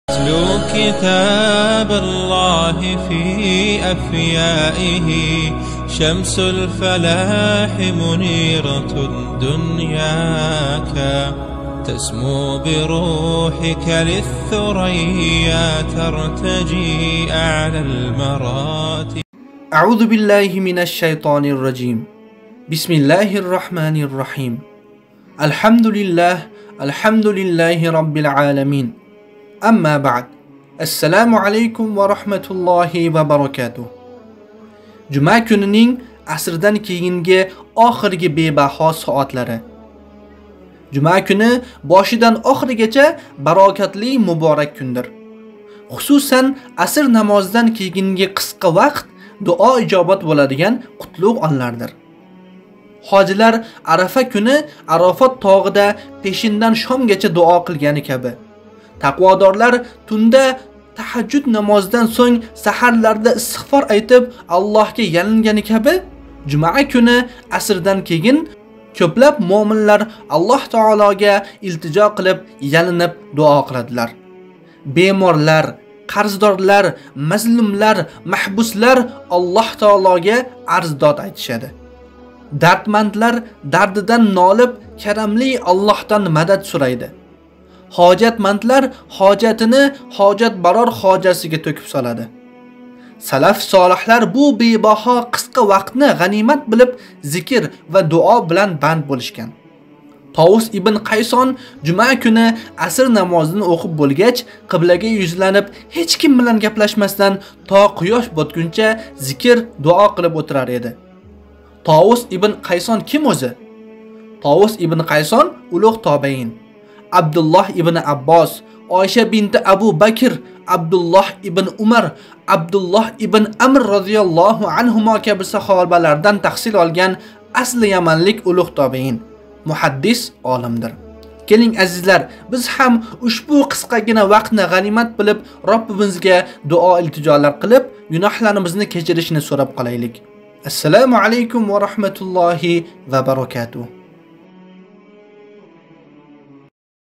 ҚАРЗДОРЛАР, МАЗЛУМЛАР, БЕЪМОРЛАР СИЗЛАР УЧУН ТИНГЛАНГ.. | дуолар канали
ЖУМАНИНГ АСРДАН КЕЙИНГИ ОХИРГИ БЕБАҲО СОАТЛАРИ

Жума куни бошидан охиригача баракотли муборак кундир! Хусусан асрдан кейинги қисқа вақт дуо мустажоб бўладиган қутлуғ онлардир.

Ҳожилар Арафа куни Арафотда пешиндан шомгача дуо қилгани каби, тақводорлар тунда таҳажжуд намозидан сўнг саҳарларда истиғфор айтиб Аллоҳга ялингани каби жума куни асрдан кейин жумла мўминлар Рабб таолага илтижо қилиб ялиниб-ёлворадилар.

Беморлар, қарздорлар, мазлумлар, маҳбуслар Роббул оламийнга арз-дод айтишади.

Дардмандлар дардидан нолиб карамли Аллоҳдан мадад сўрайди.
Ҳожатмандлар ҳожатини ҳожатбарор хўжасига тўкиб солади.

Салафи солиҳлар бу бебаҳо қисқа вақтни ғанимат билиб зикр ва дуо билан банд бўлишган.

كان طاوس بن كيسان إذا صلى العصر من يوم الجمعة استقبل القبلة، ولم يكلّم أحدًا حتى تغرب الشمس.

Товус ибн Кайсон Жума куни аср намозини ўқиб бўлгач, қиблага юзланиб, ҳеч ким билан гаплашмасдан то қуёш ботгунча зикр-дуо қилиб ўтирар эди.

Товус ибн Кайсон ким?

Товус ибн Кайсон улуғ тобеин.

Абдуллоҳ ибн Аббос,
Оиша бинт Абу Бакр,
Абдуллоҳ ибн Умар,
Абдуллоҳ ибн Амр розияллоҳу анҳум каби саҳобаларда таҳсил олган асли яманлик улуғ тобеин, муҳаддис олимдир.

اللهم إنا نسألك الهدى والتقى والعفاف والغنى

اللهم صل و سلم و بارك وزد و أنعم على عبدك و نبيك محمد صلى الله عليه وسلم

Жалол Иброҳим.